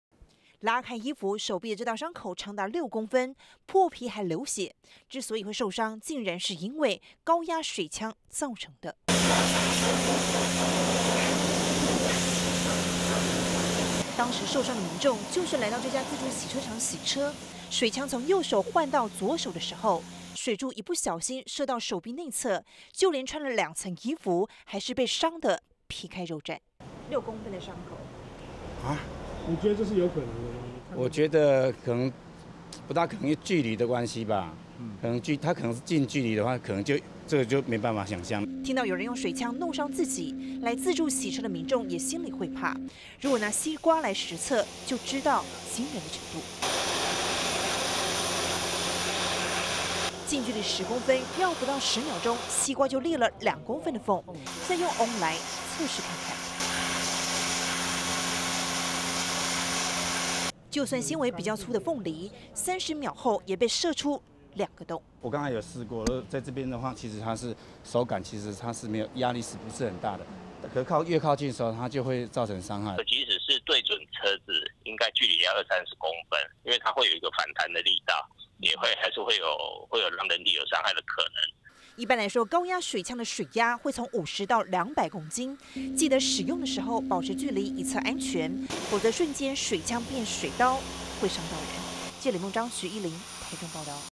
20170310中天新聞 自助洗車場藏利器！ 高壓水槍射傷手臂
自助洗車高壓水槍竟成傷人利器！台中市有位先生，到自助洗車場洗車，使用高壓水槍時，不小心不射到自己的左手臂內側，這一射竟然皮開肉綻，直說太恐怖！我們拿了西瓜和鳳梨實測，發現高壓水槍，竟然會將兩種水果都打爛，專家提醒，洗車用高壓水槍水柱壓力最高可達每平方公分200公斤，最好保持20公分以上的距離！